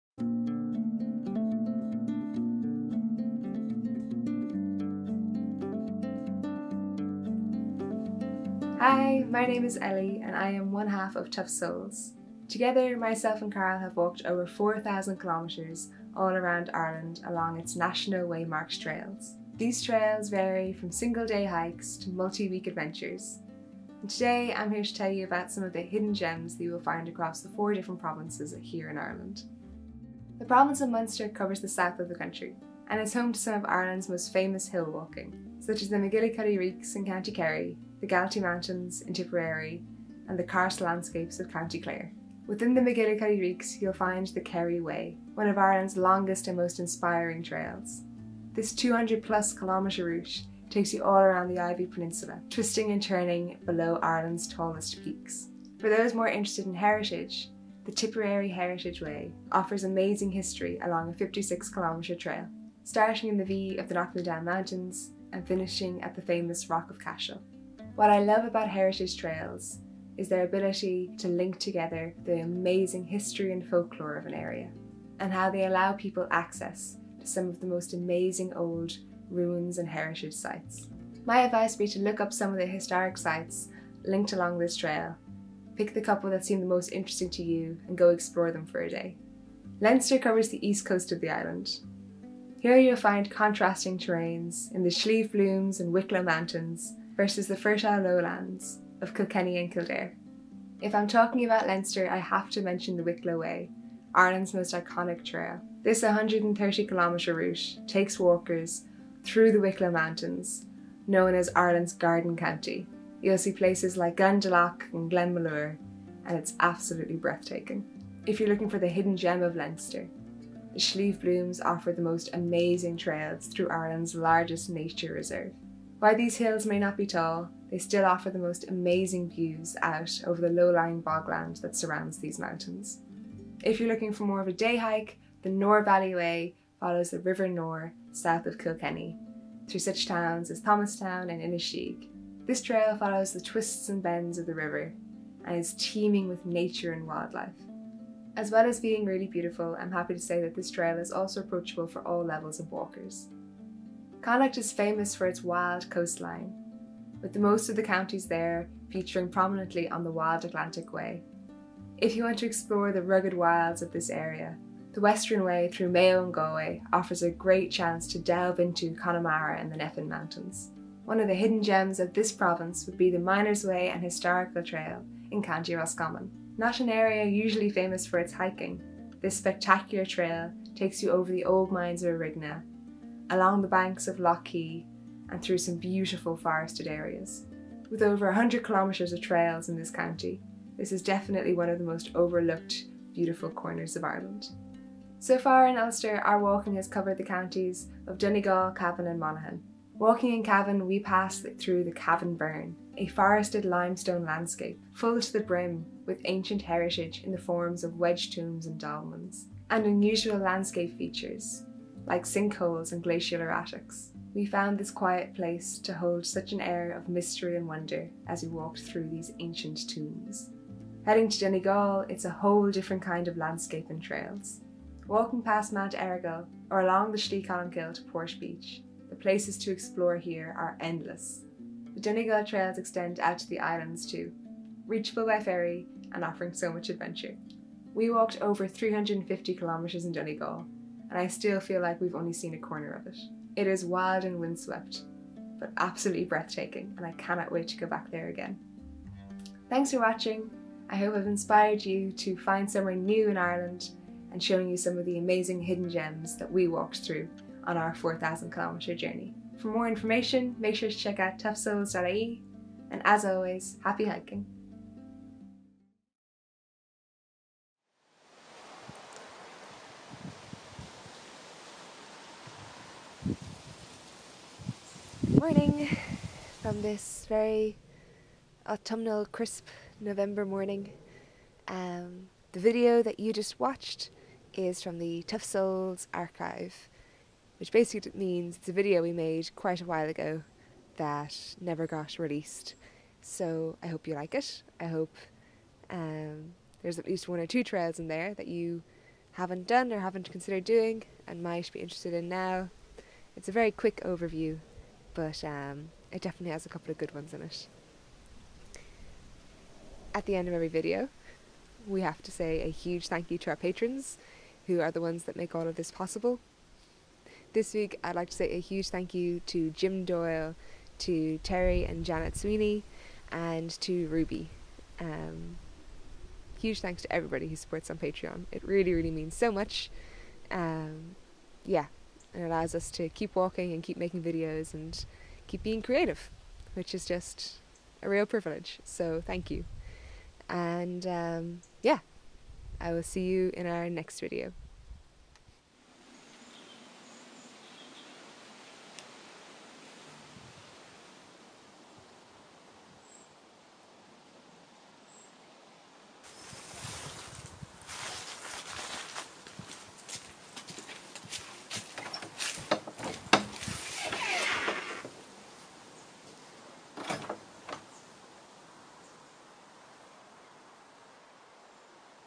Ireland's Hidden Hiking Gems 👣 Tough Soles
This is a quick video sharing some of the amazing hiking you can find in the four provinces of Ireland. For each province we've mentioned one trail that you've probably heard about, and one of the hidden gems in that corner of Ireland!

☀︎ TRAILS MENTIONED ☀︎
Munster:

We are Ambassadors with GreatOutdoors.ie, who help us figure out the best gear for our adventures! Thanks to Great Outdoors for their continued support